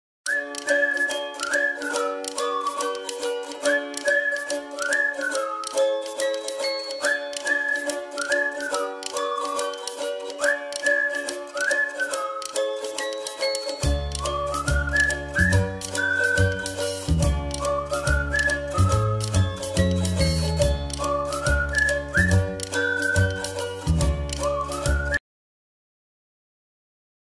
flamingo#short#fishing
would you like flamingo ...then here it is for you a lovely video of your favourite Flamingo..at riverside searching delicious fishes..along with beautiful family... really it's nice video to watch..